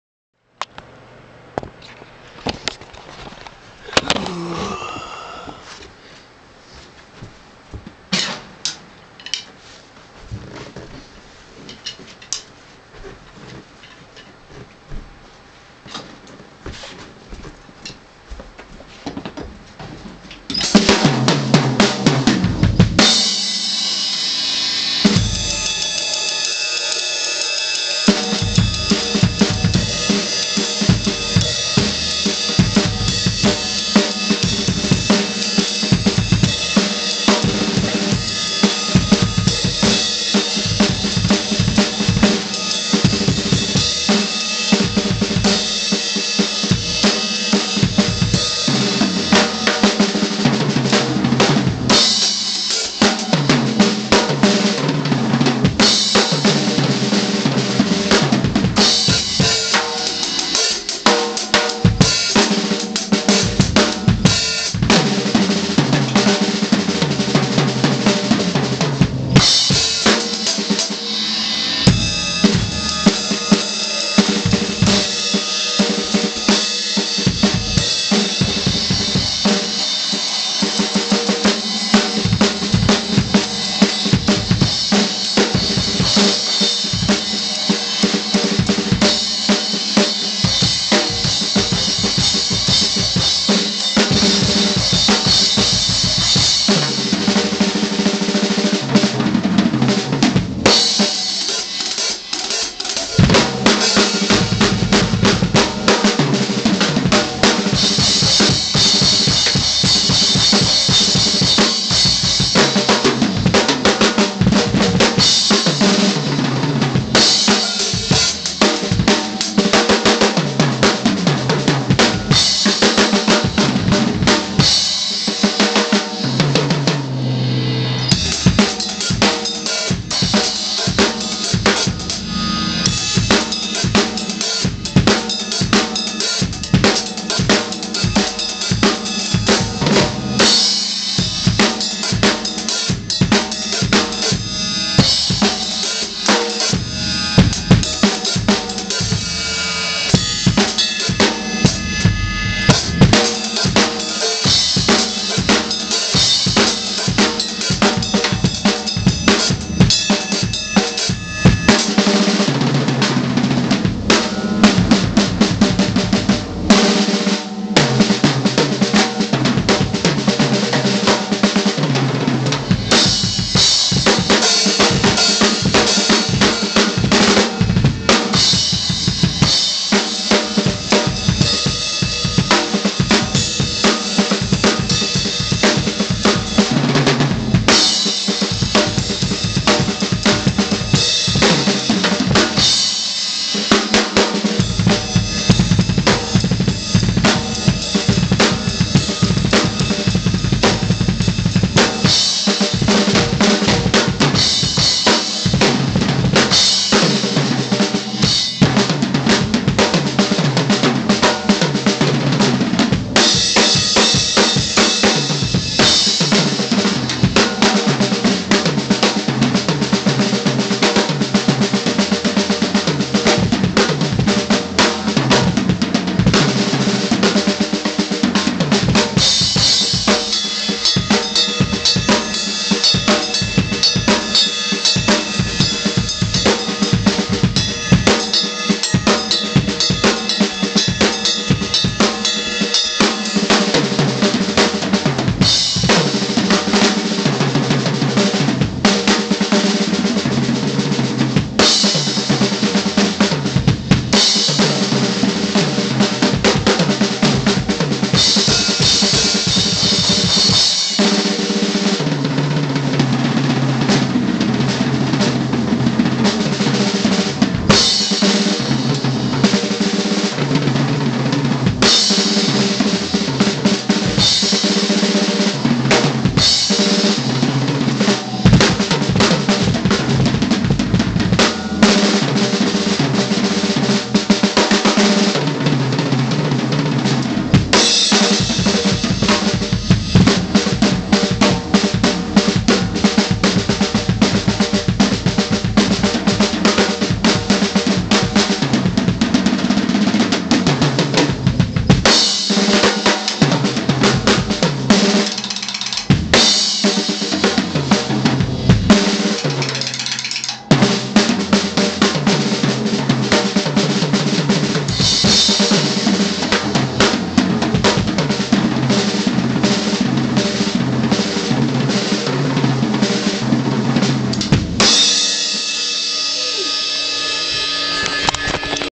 Drum Solo #3
i hate double bass pedal dont be a faggot im working on it

just doing some more stuff,...grooves and things..

tell me if you like

im just messing around so dont say all that degrading shit that usual people say because they think they are fantastic when they really blow ass

any real drummer or musician or any person for that matter would always want to support any talented or learning musician

unless your jealous
then you would say something gay or douchey and no one likes you

if you are good you are good

if you suck, your true friends will give you moral support and you will have the will to keep practicing and become savage